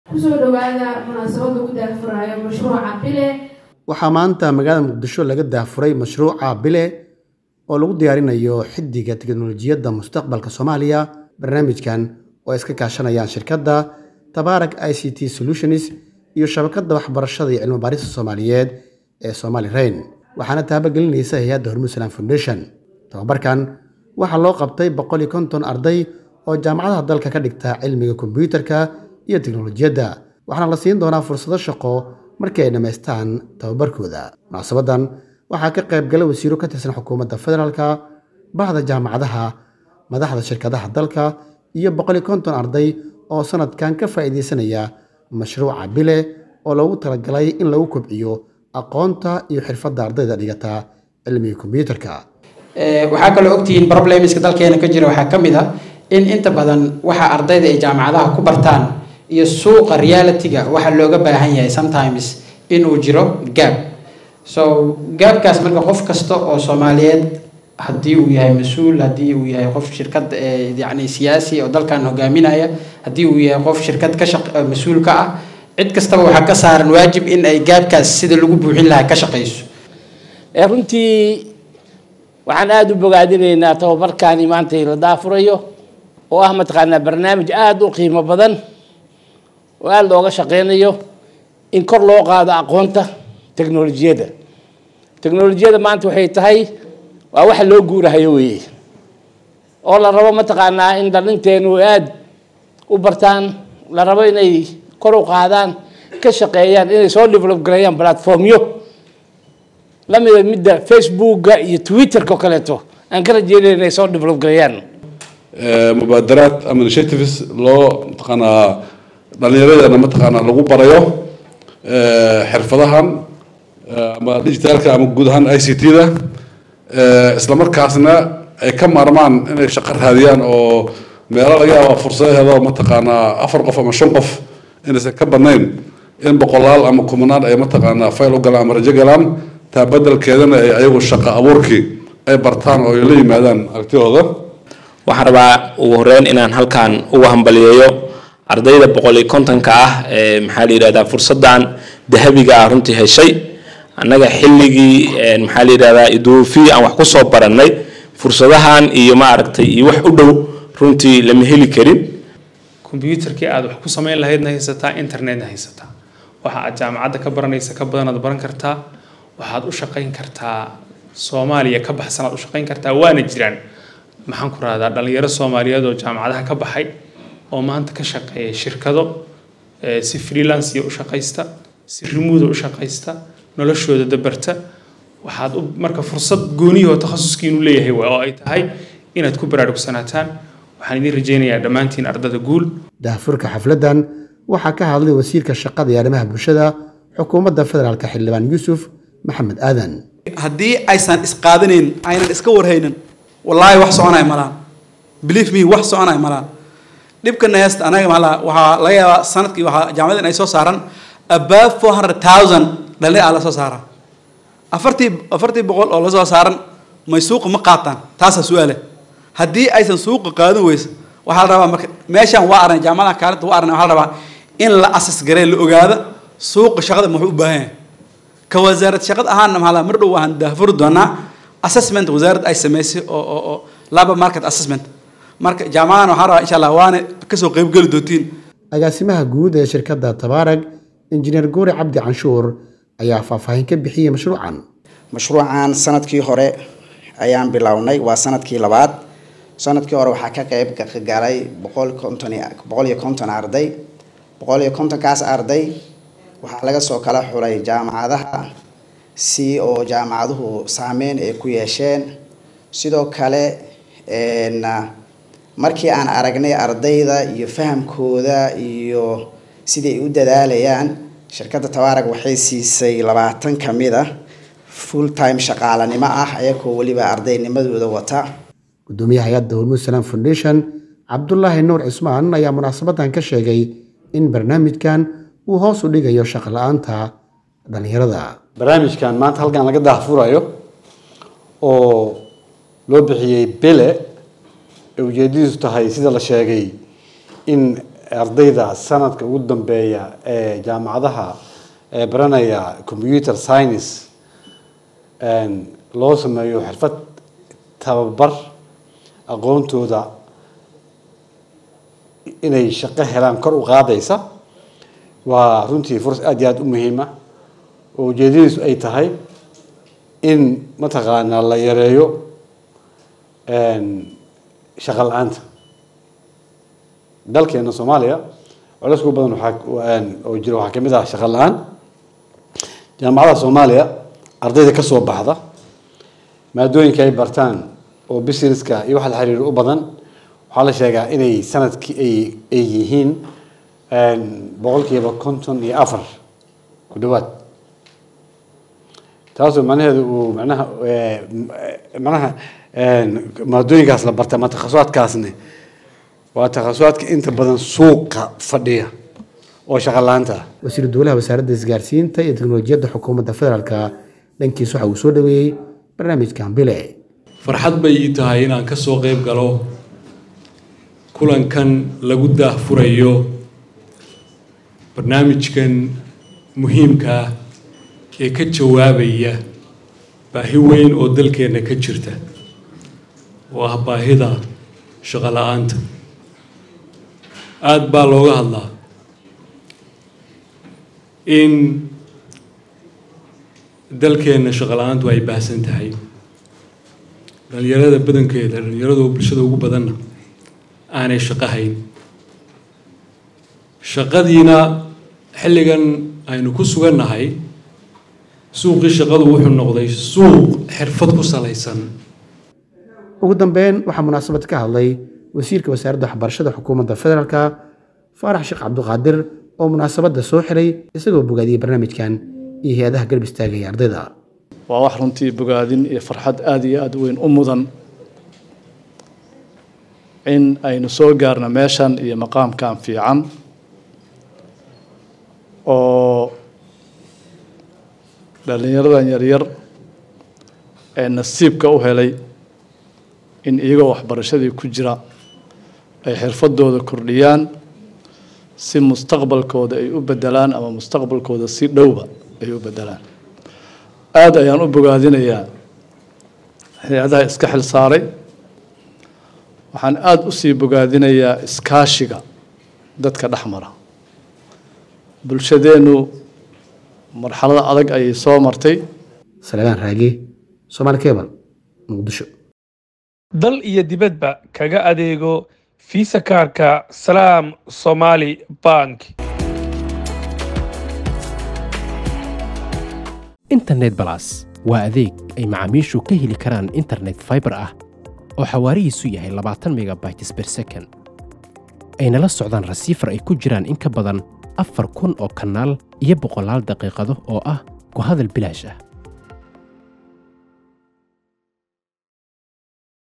XIDIGAHA TIKNOLOJIYADA:-Muqdisho oo laga daahfuray Mashruuca Bile oo lagu diyaarinayo Xidigaha...
XIDIGAHA TIKNOLOJIYADA:-Muqdisho oo laga daahfuray Mashruuca Bile oo lagu diyaarinayo Xidigaha Tiknolojiyada Mustaqbalka Soomaaliya.